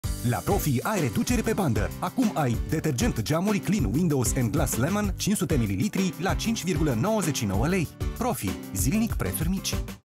La Profi ai reduceri pe bandă: Detergent geamuri Clin windows & glass lemon 500ml la un pret special
La Profi ai reduceri pe bandă! Acum ai Detergent geamuri Clin windows & glass lemon 500ml la un pret special. Oferta supusa unor termene si conditii. Profi, zilnic preturi mici